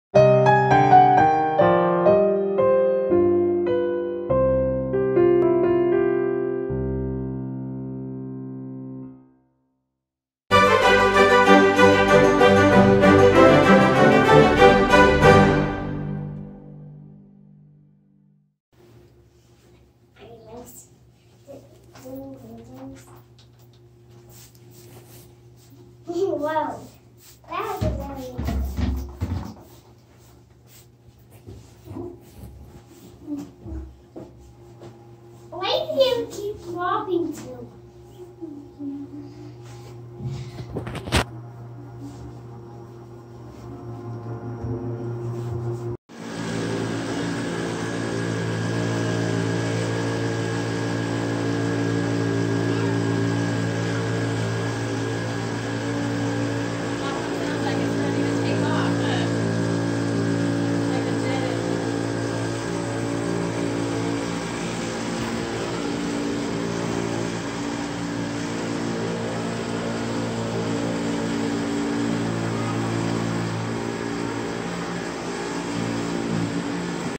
Little Maids
We have a service opportunity to clean a building near our home on occasion. The kids are such great helpers, that I had to brag just a bit about how proud I am of them!